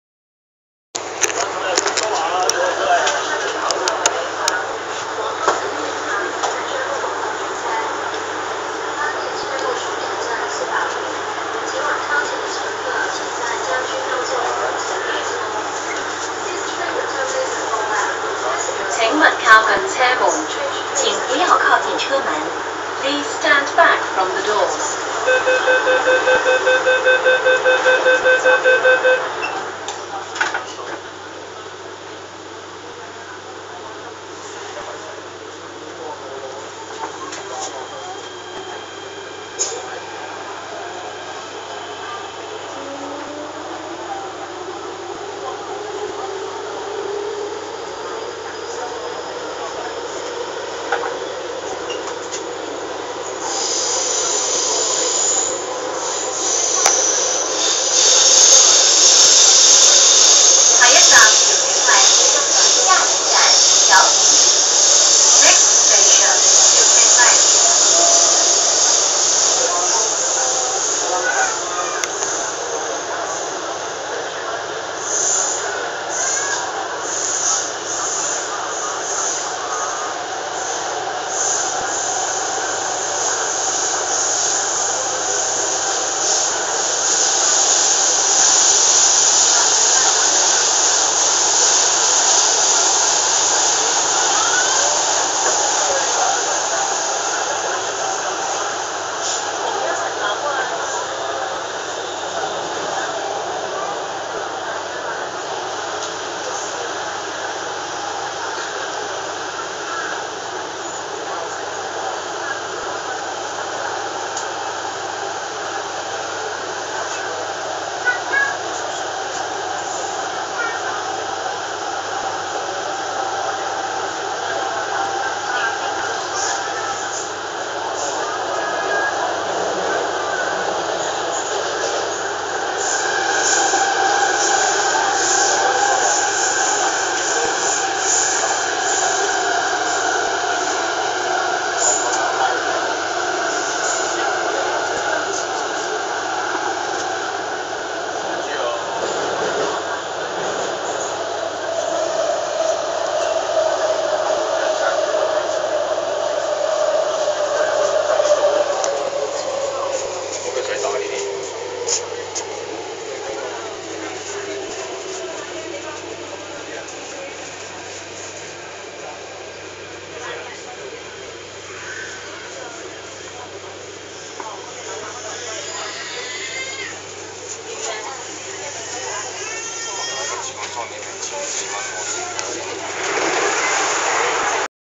Rding On MTR Tseung Kwan O Line K-Train C304 From Yau Tong To Tiu Keng Leng
Train:A303-A304(car:C304)
Tag:港鐵Rotem電動列車 Rotem電動列車 港鐵 將軍澳綫 MTR Tseung Kwan O Line K-Train K-stock train